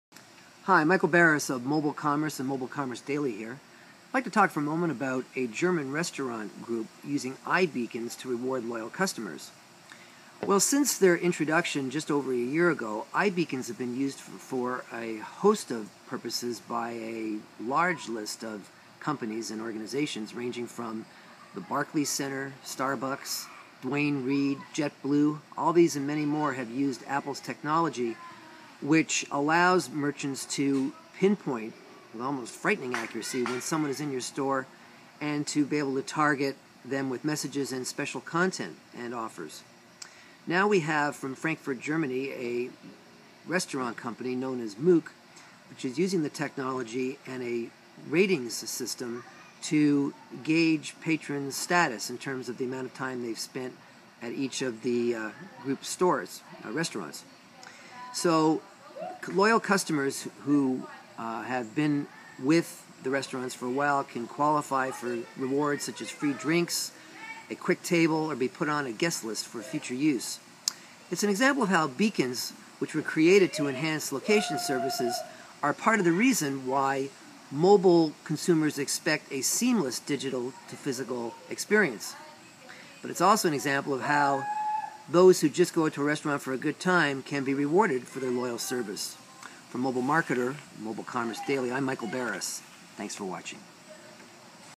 German restaurants’ iBeacons tryout rewards loyal customers
An upscale German restaurant group’s testing of Apple’s iBeacon technology is streamlining the in-store experience with a mobile application that rewards loyal customers for spending time there.